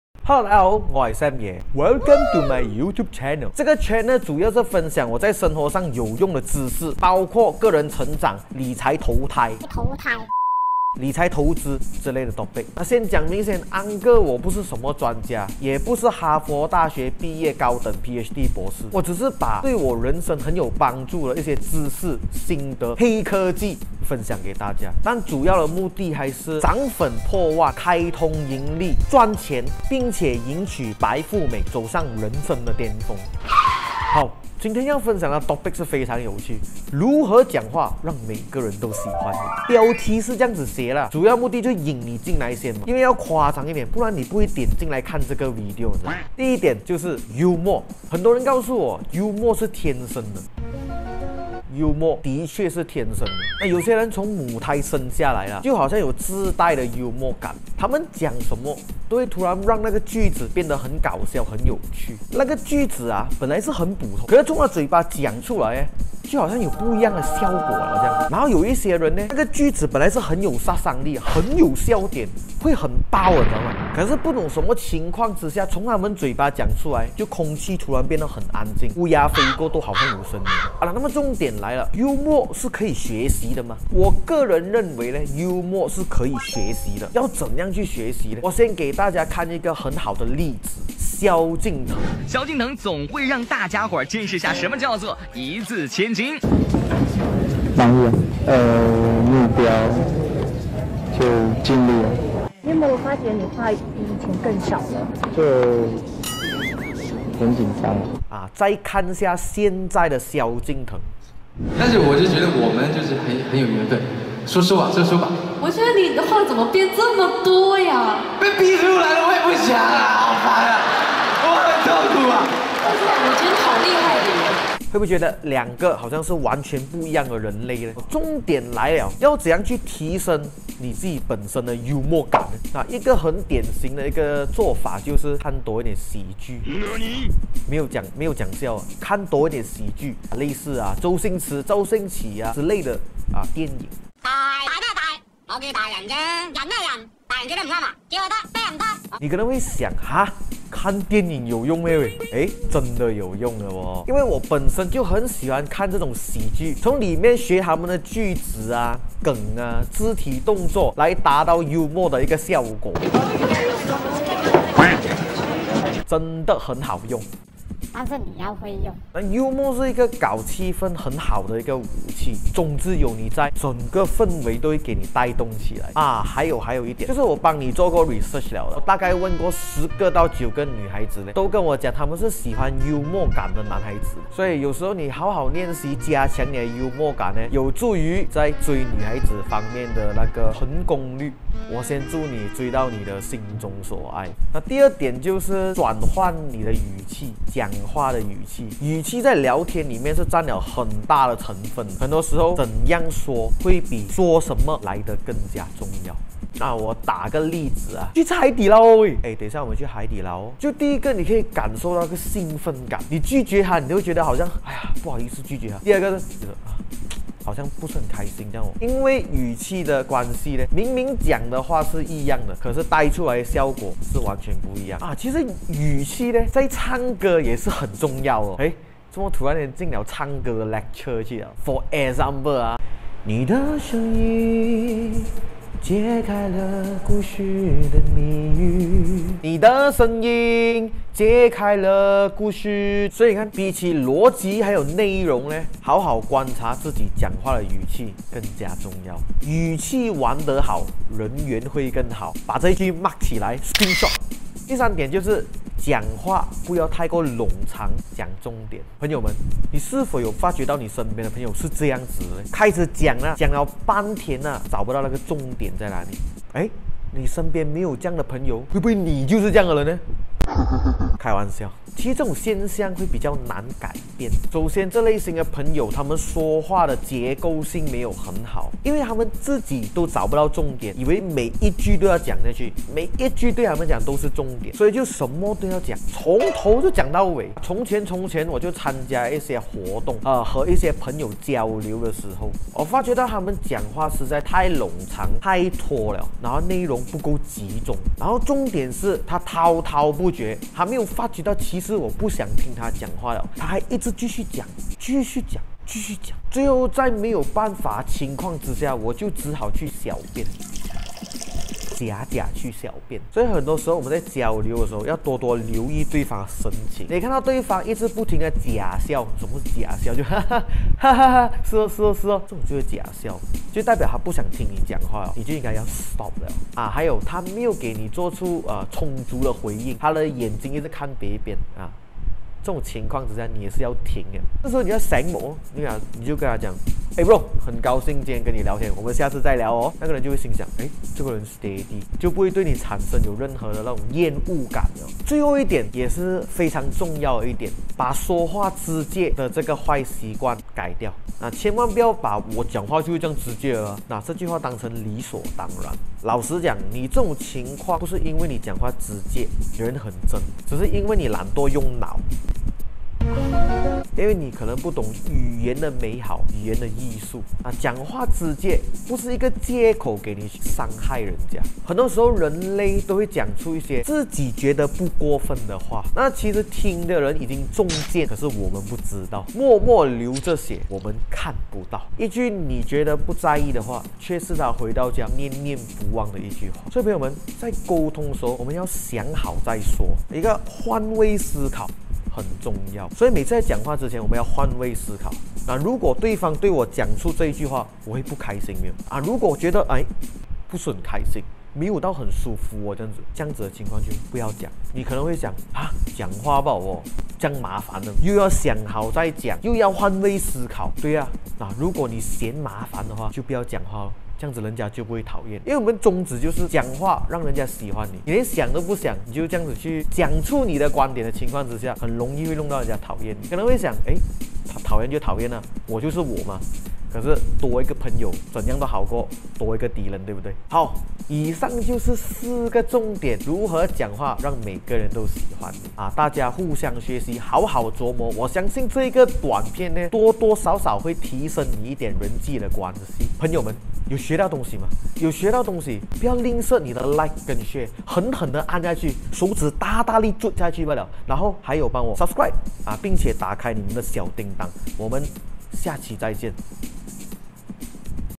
聊天技巧 | 如何说话让每个人都喜欢你 | 超有效提升人际关系的说话方法！幽默是聊天的绝佳武器，转换语气会带来不同的效果 | 改掉说话直接的坏习惯！社交也要有技巧【Sam爷讲堂】
聊天技巧 | 如何说话让每个人都喜欢你 | 4种提升人际关系的说话方法！幽默是聊天的强大武器，转换语气带来不同的效果，社交也要有技巧。